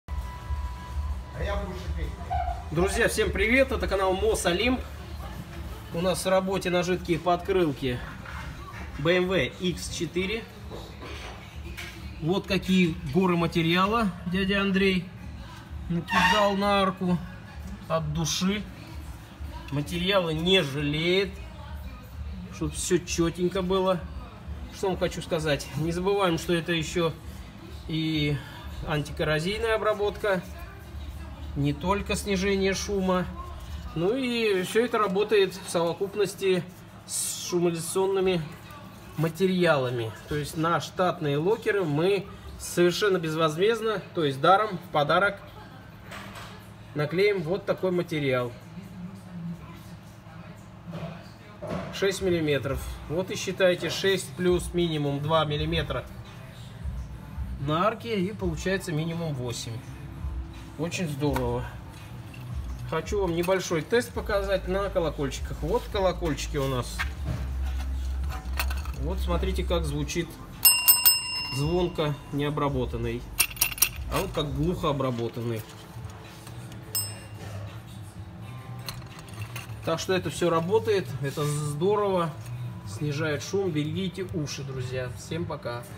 BMW X4 идеальный вариант защиты арок от коррозии и повреждений - и это жидкие подкрылки
Работаем с BMW X4 - будем делать процедуру, которую обычно заказывает подавляющее большинство автовладельцев, вне зависимости от того, какая у них машина. Это жидкие подкрылки - идеальная защита от коррозии на колесных арках, а заодно и наружная шумоизоляция. Обилие агрессивных противогололедных реагентов на столичных улицах делает эту процедуру обязательной для всех и каждого, ведь резинобитумное покрытие - это одна из немногих защитных мер, которые действительно способны противостоять "соли" с грязных дорог. Ну а что касается шумоизоляционного эффекта - он здесь не такой уж и небольшой, как может показаться, и мы даже можем продемонстрировать вам его вживую - смотрите видео и оценивайте!